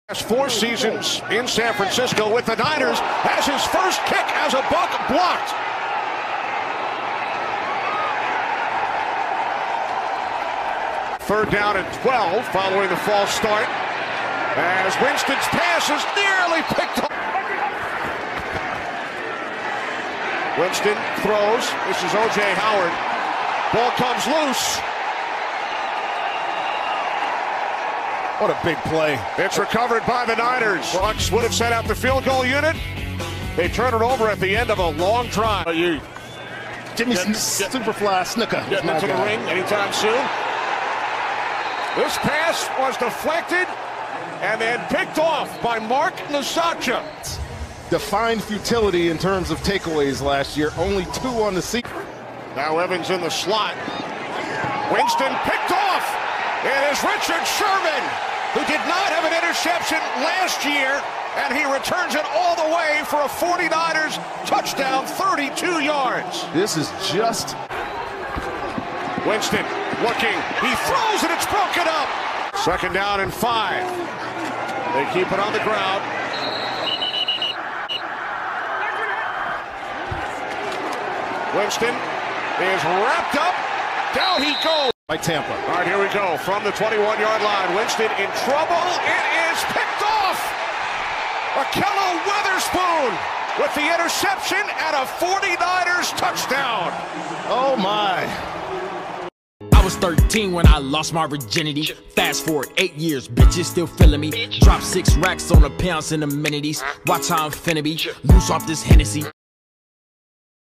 49ers Defense VS Buccaneers
2 fumbles, 2 touchdowns, 3 interceptions 8 deflections.